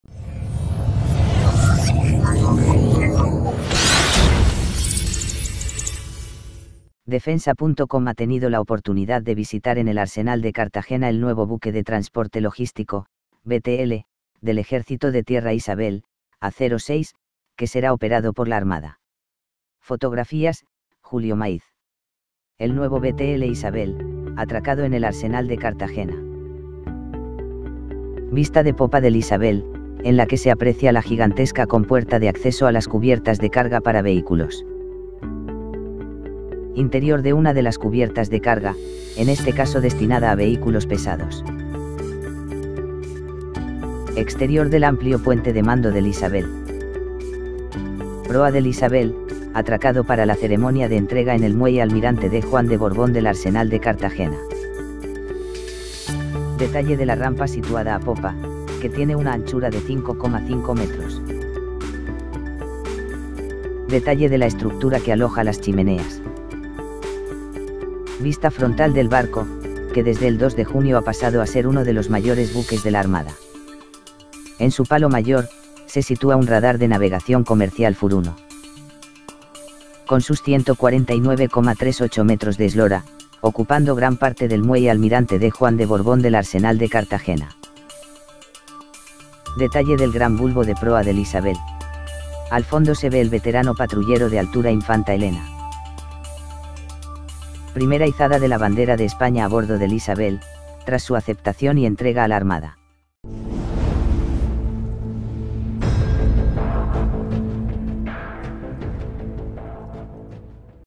Visitamos el "nuevo" buque de transporte logístico del Ejército español “Ysabel”
defensa.com ha tenido la oportunidad de visitar en el Arsenal de Cartagena el “nuevo” buque de transporte logístico (BTL) del Ejército de Tierra (ET) “Ysabel” (A-06), que será operado por la Armada. (fotografías: Julio Maíz)

Defensa.Com